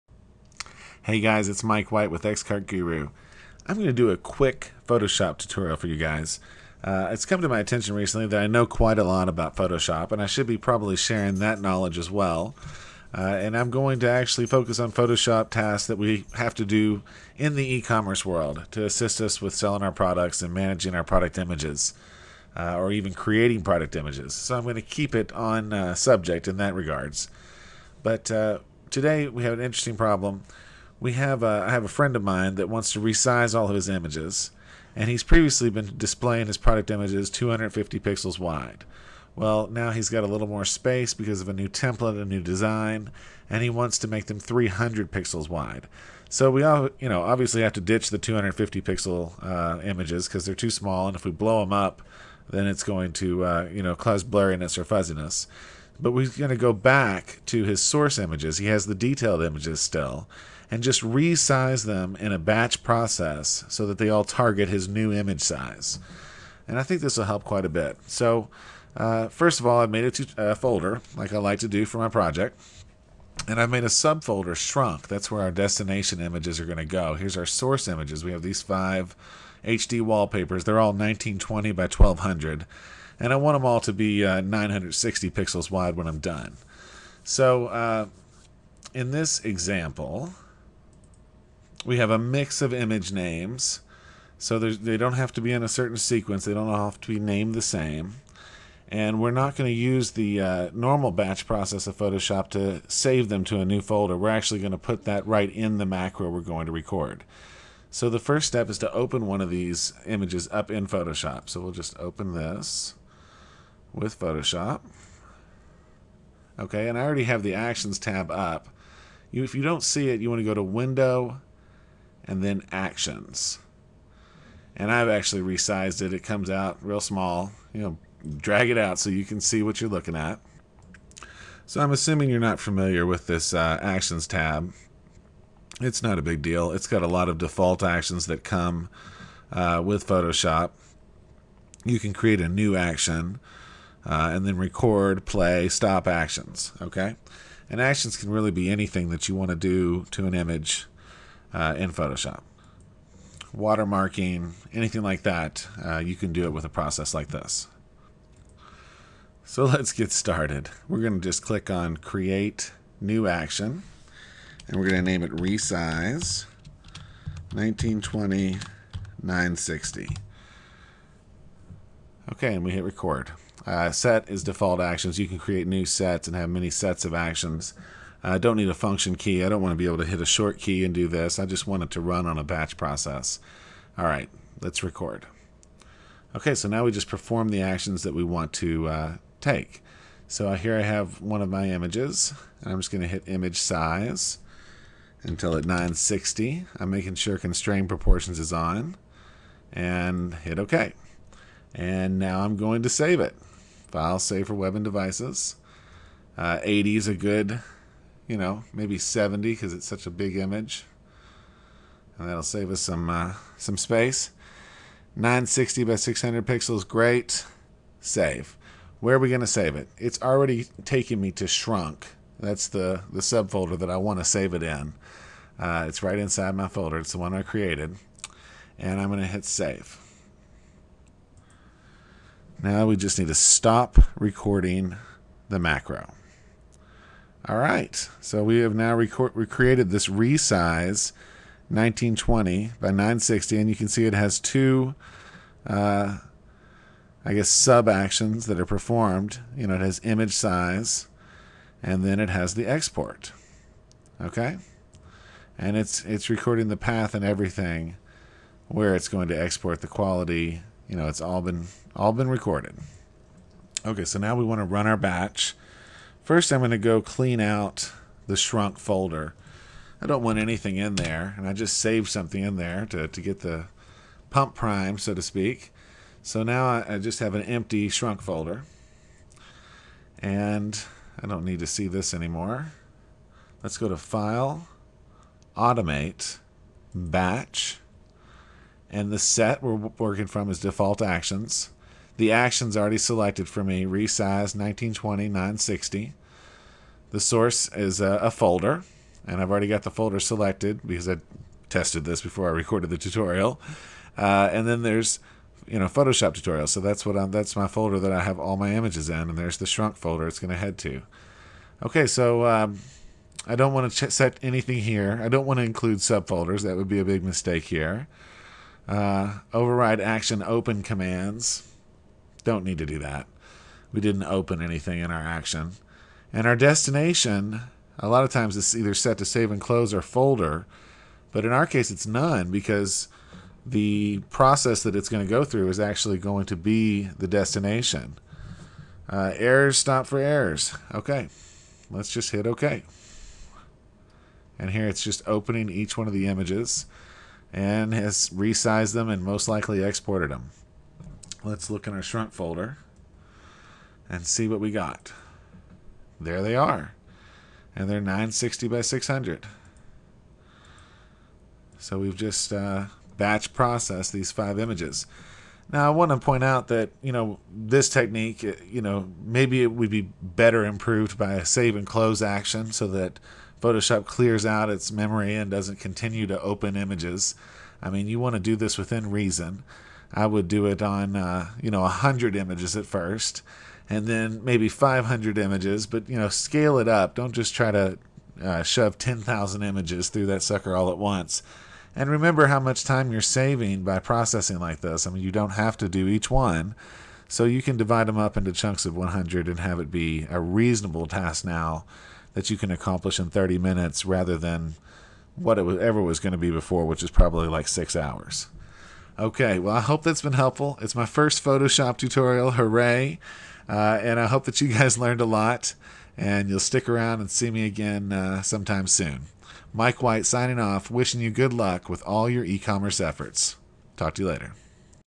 Batch Process Product Photos with Photoshop
we learn how to record macros and batch process an entire folder of images with Photoshop. Perfect for re-sizing those product thumbnails...